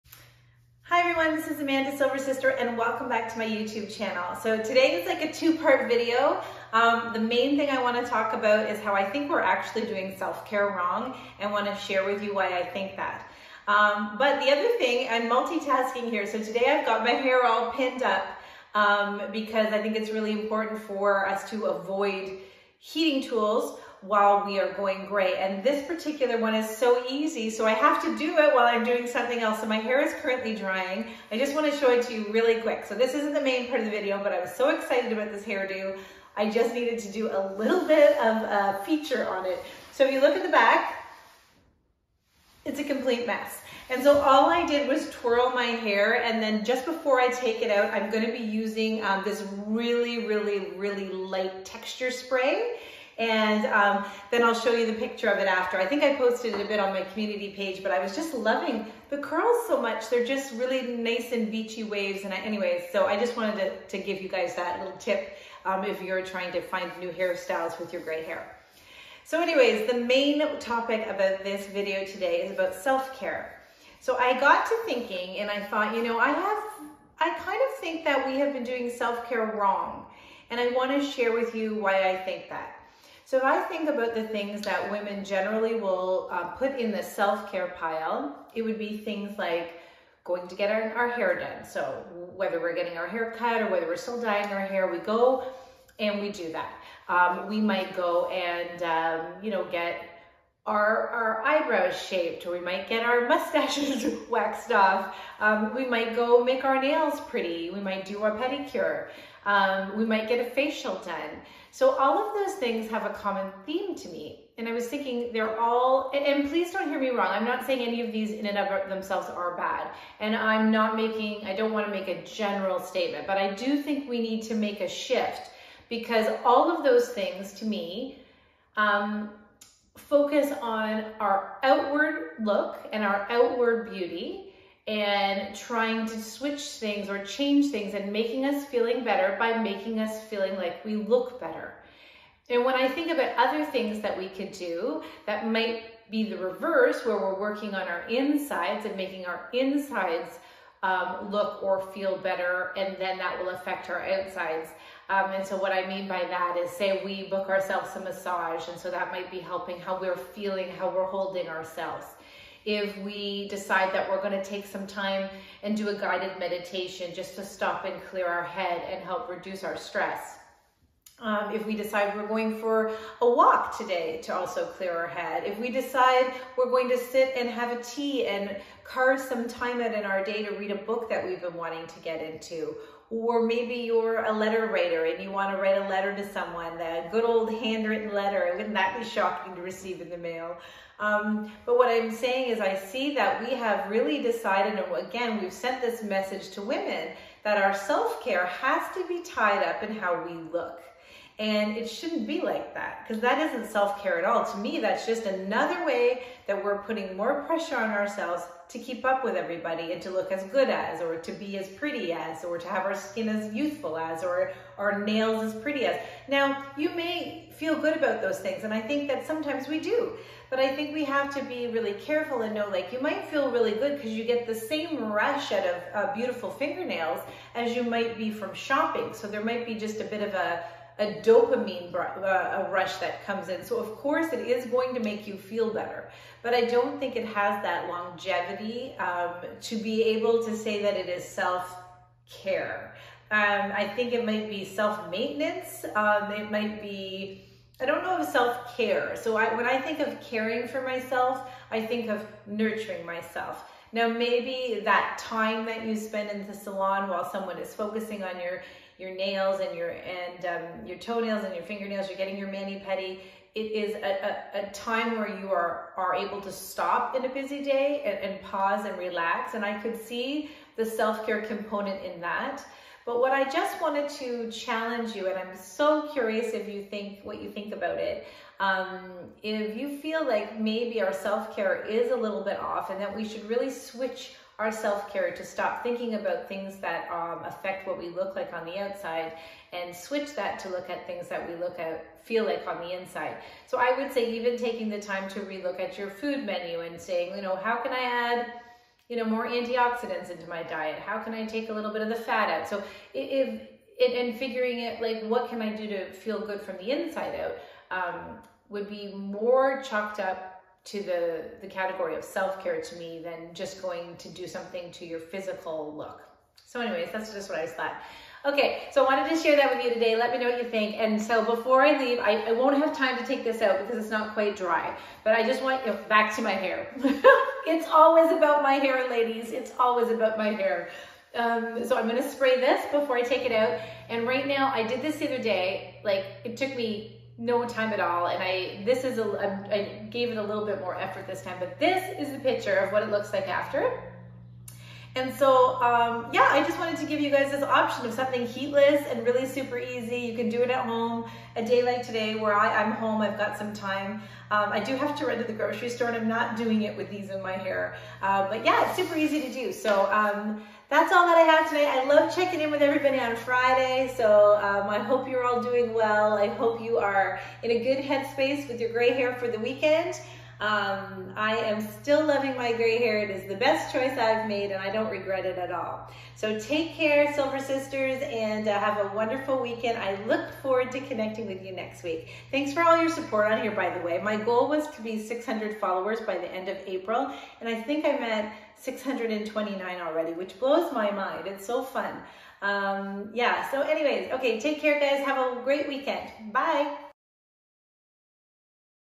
I think we all have been doing our self care wrong!! Let me tell you why!!
Have you noticed that our self care is often focused on improving our outward appearance? Well I think this might be the wrong approach to self care…
Oh and I quick hair tutorial… lol!